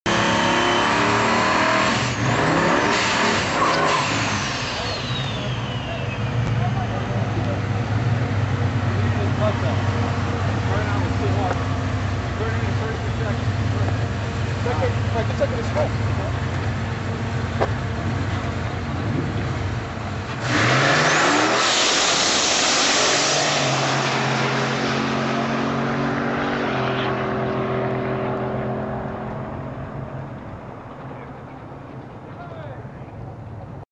MM&FF x project car Hooligan Hot Rod 347C.I turbocharged
listangs.com track rental MM&FF x project car 347 C.I Turbocharged. For more info on the car go